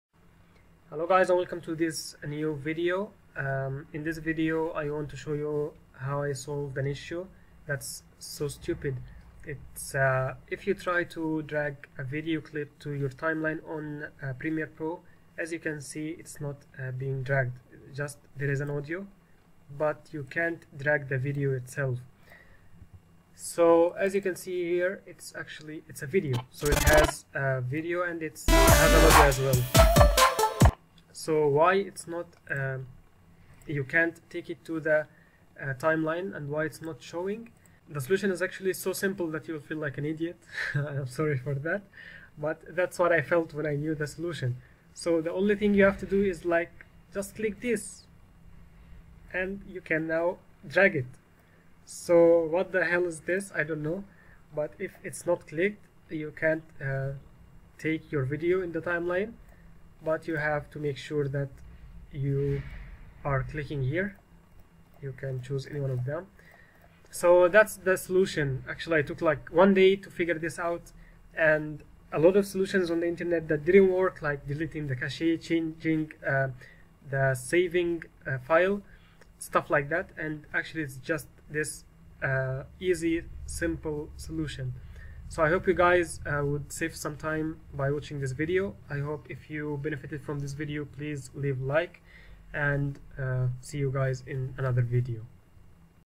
Can't drag Video to Timeline on Premiere Pro Problem Solved !
Why cant you drag video into your timeline? Well, here is the solution :D